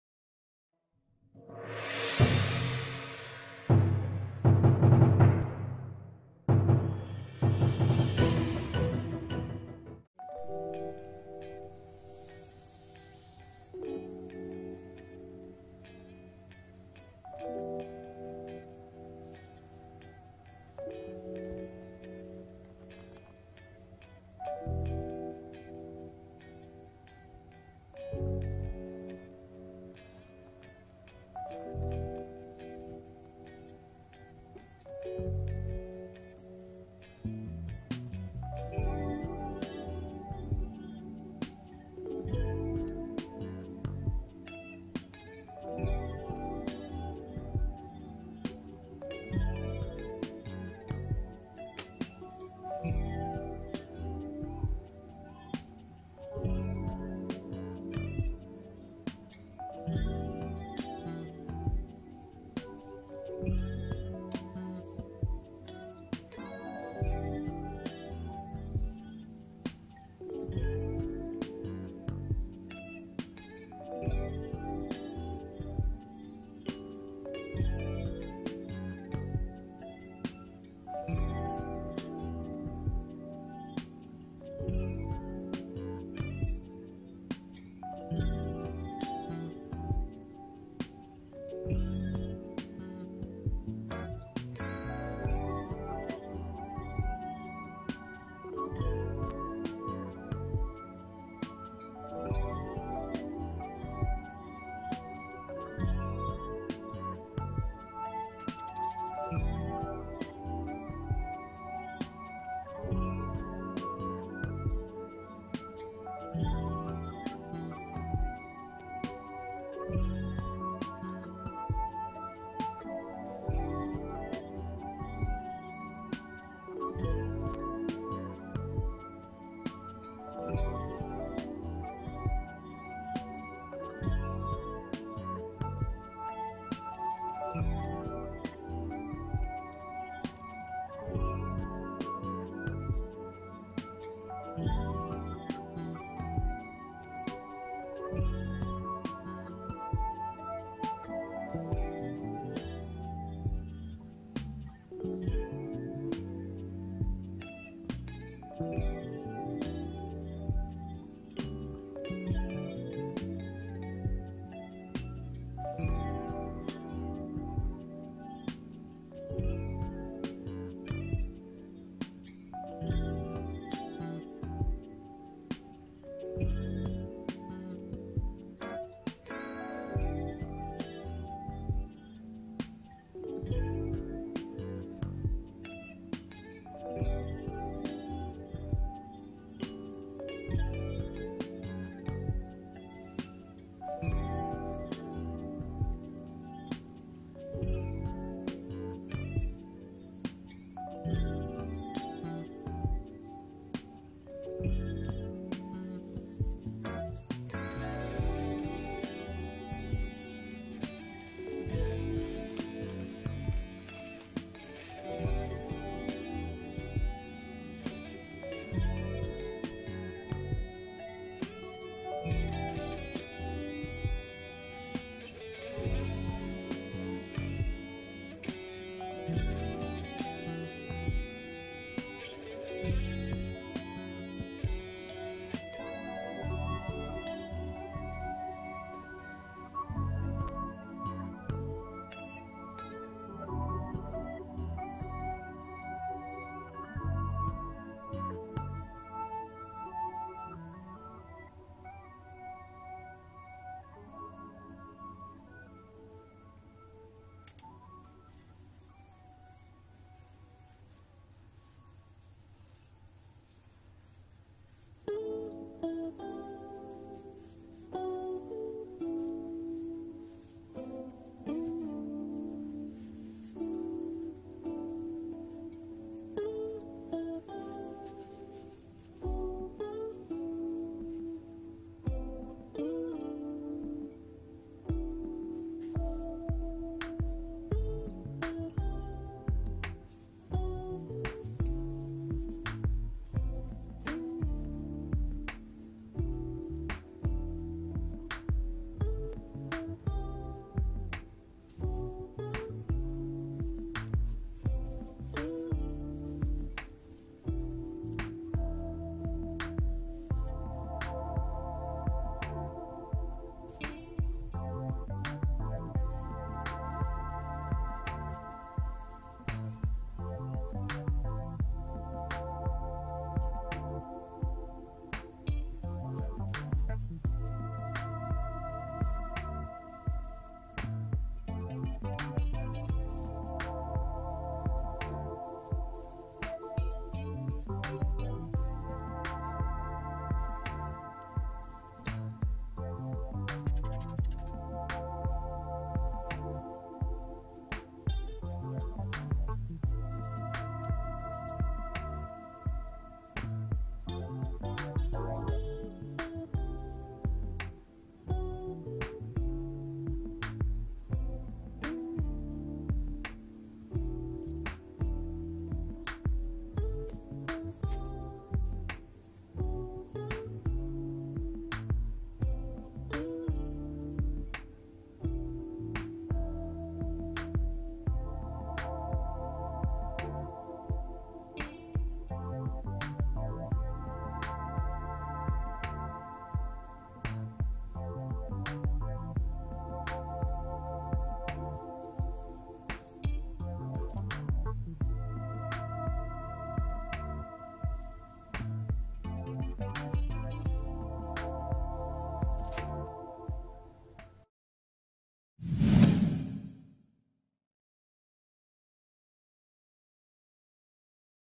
How to Remove Chrome From Plastic Parts
Remove Chrome From Plastic Parts

Method to removing chrome plating on a model kit plastic parts. Just soaking them in regular bleach.